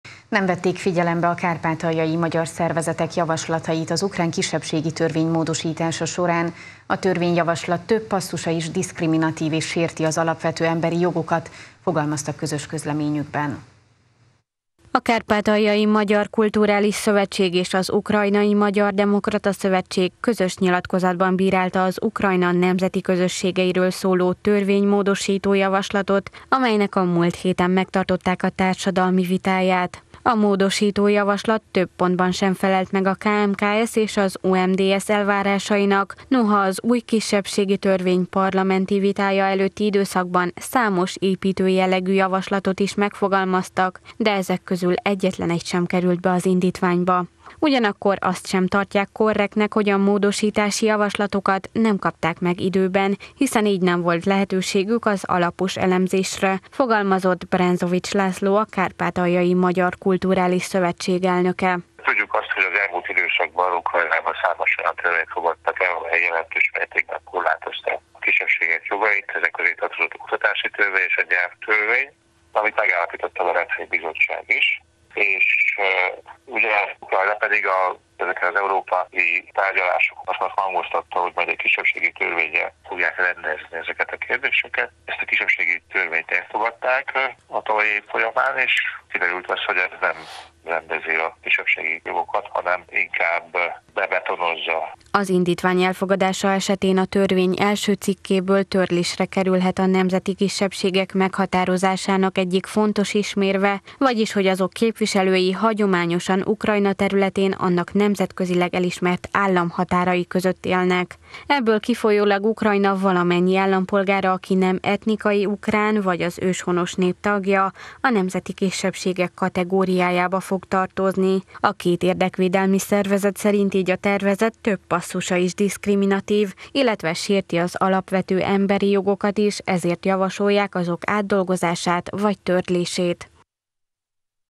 Nem elégedettek a kisebbségi törvénnyel – Erdélyi Magyar Televízió
Nem vették figyelembe kárpátaljai magyar szervezetek javaslatait az ukrán kisebbségi törvény módosítása során. A törvényjavaslat több passzusa is diszkriminatív és sérti az alapvető emberi jogokat – fogalmaztak közös közleményükben.